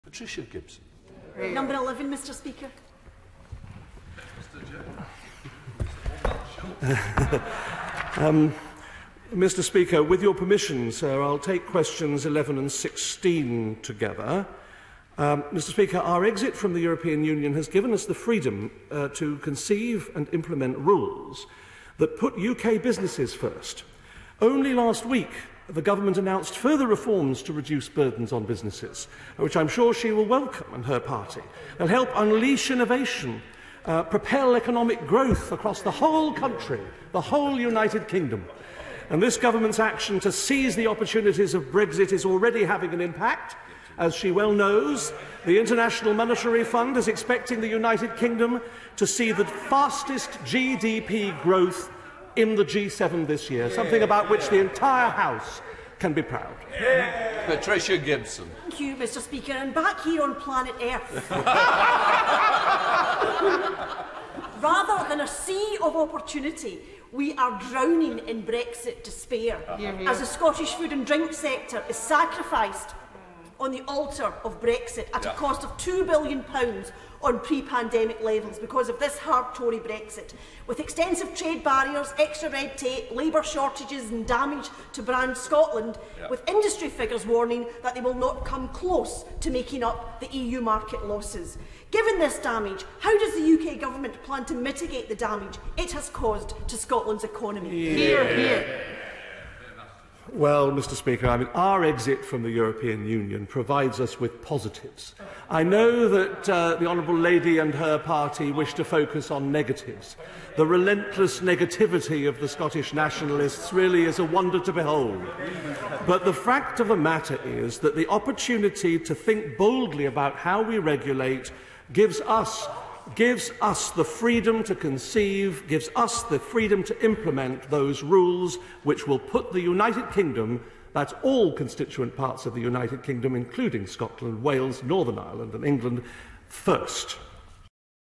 TORIES DELUDED AS HARSH REALITY OF BREXIT BITES
Scotland’s food and drink sector is being scarified on the altar of Brexit, with new trade barriers, extra red tape, labour shortages and damage to the Scottish food and drink brand. £2 billion has been lost by the sector in comparison to pre-pandemic levels, and industry leaders are warning that it is not possible to make up EU losses.

I therefore asked the UK Government how it plans to mitigate the damage caused to Scotland’s economy by the Tories’ dangerous and ideological obsession with a hard Brexit.

Sadly, it is clear that the UK Government would rather engage in fantasies about the mythical opportunities Brexit provides than practical support for struggling Scottish businesses